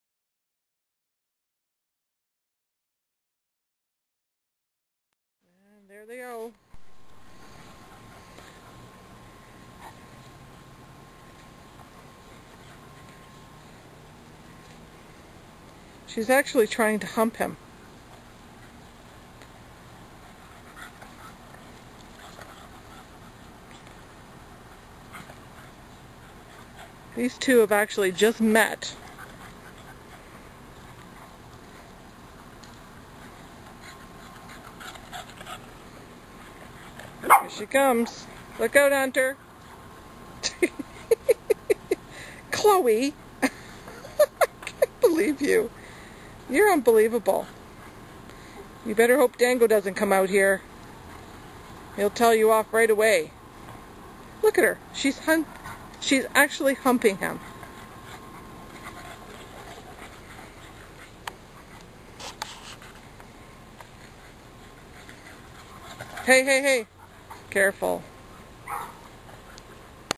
Chloe and Hunter 2.wmv
Chloe the Boston terrier mix decided to try and hump Hunter -the 110lb Bernese Mountain Dog in order to show him who is BZO